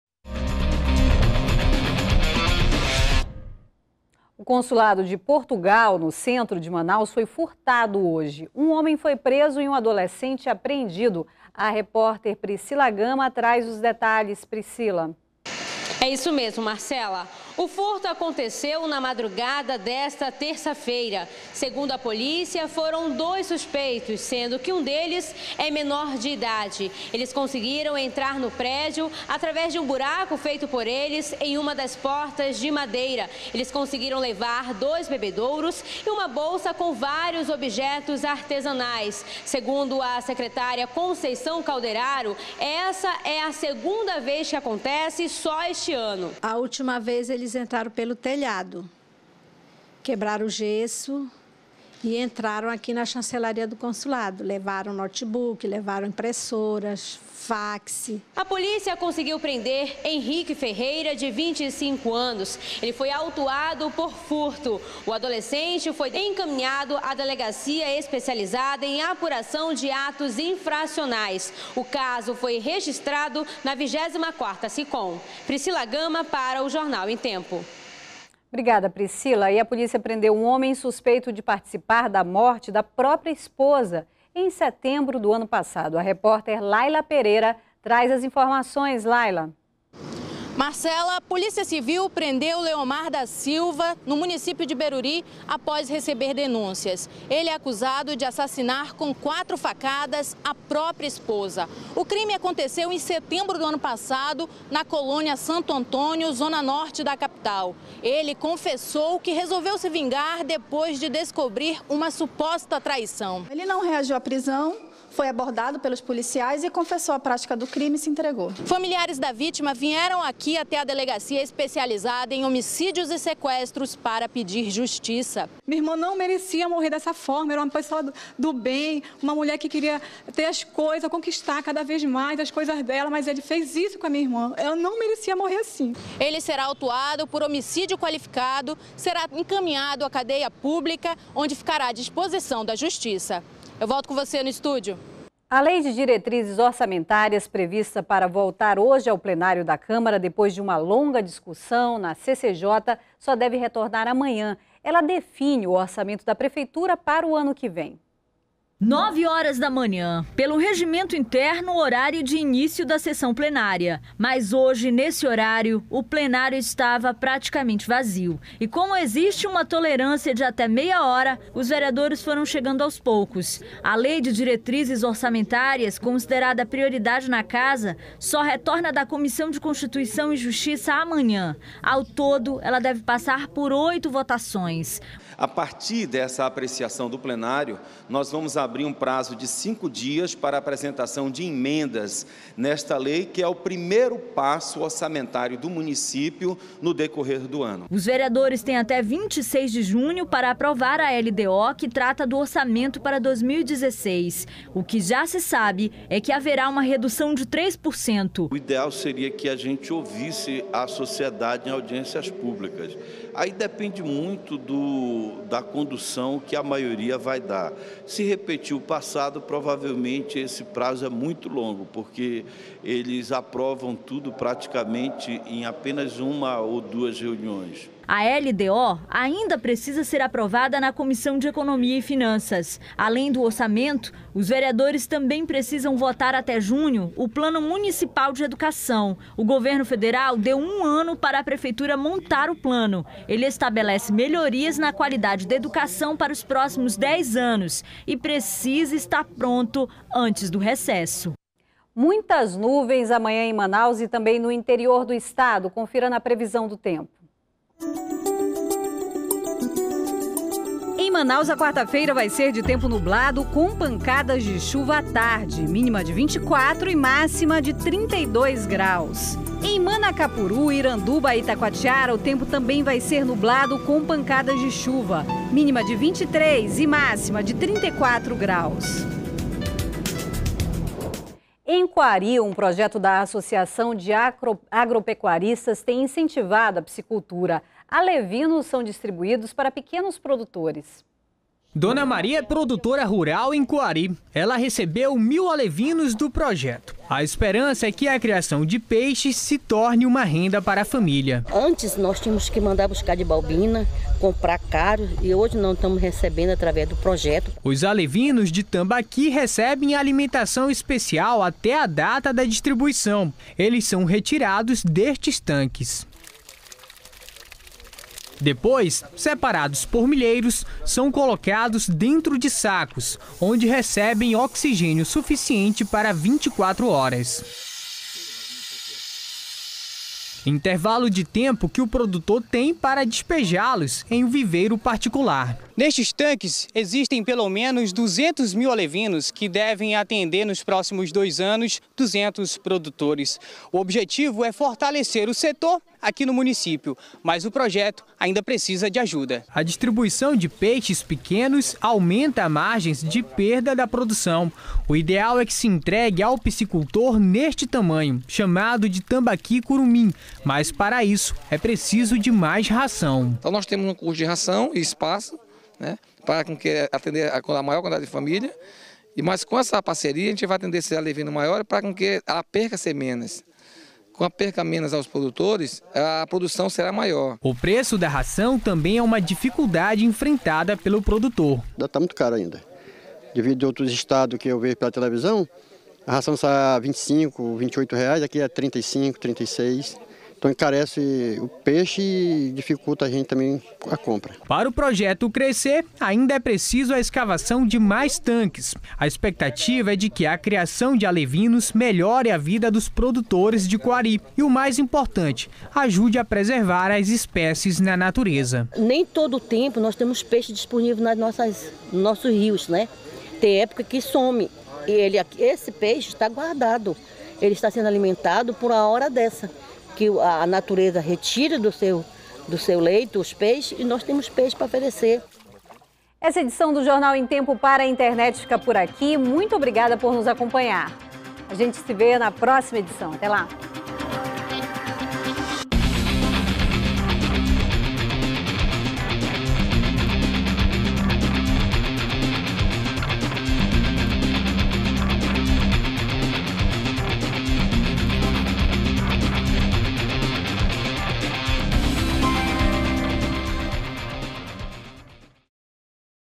Projeto tem incentivado a piscicultura em Coari com a distribuição de alevinos
03º BLOCO JORNAL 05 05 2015 - Projeto tem incentivado a piscicultura em Coari com a distribuição de alevinos